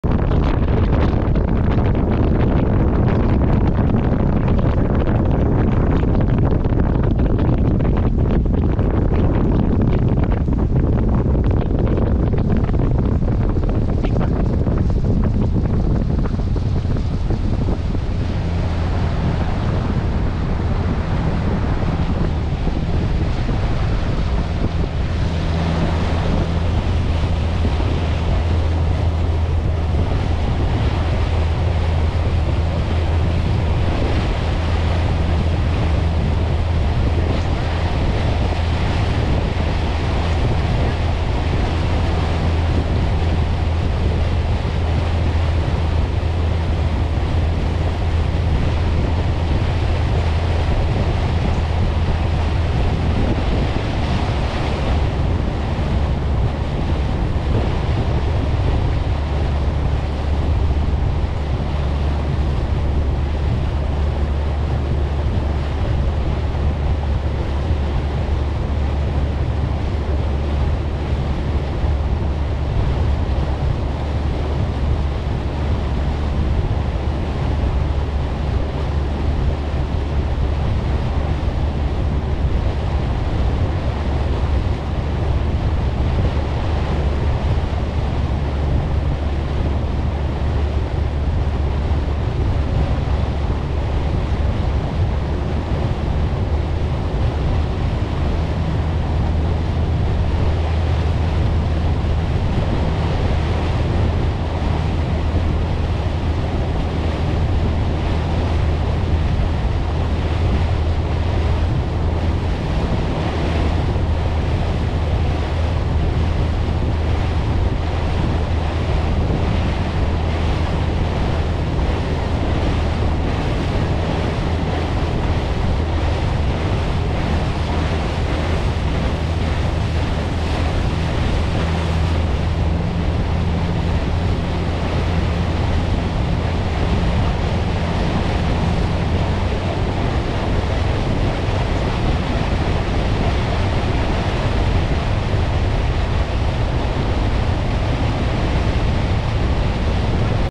Thunderstorm at Packery Channel
Thunderstorms developed in the area while we were fishing in the Laguna Madre. We even spotted a funnel cloud not far from the vessel. In this video, I am recording the island across from Clem's marina. Typically its beaches are teeming with shorebirds. I suppose that they have either evacuated or are taking shelter in the interior foliage.